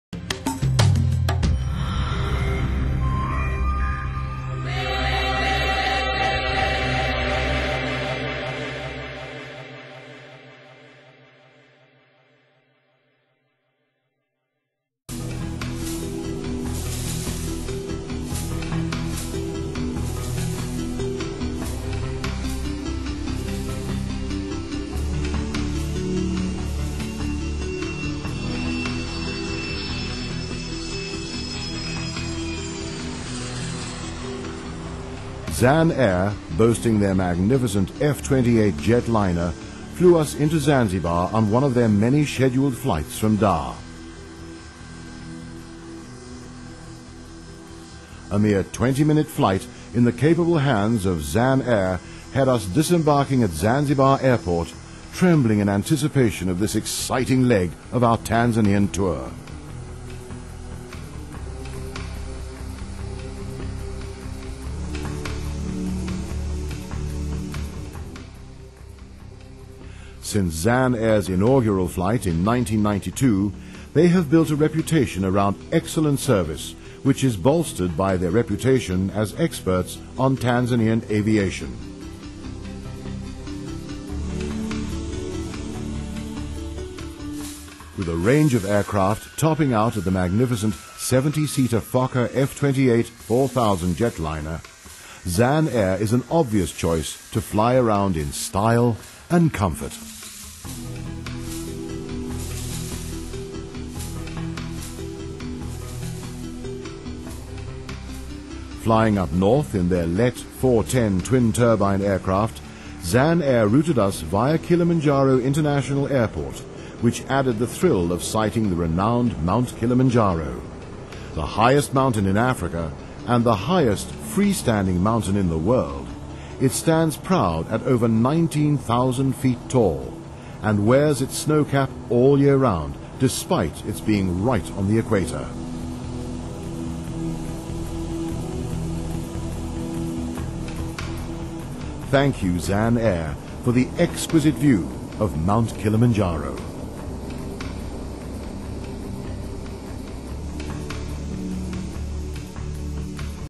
Zan Air Domestic Airline Tanzania and Zanzibar Travel - Africa Travel Channel
Since Zan Airs inaugural flight in 1992, they have built a reputation around excellent service which is bolstered by their reputation as experts on Tanzanian aviation. With a range of aircraft topping out at the magnificent 70 seater Fokker F28-4000 jetliner, Zan Air is an obvious choice to fly around Tanzania in style and comfort.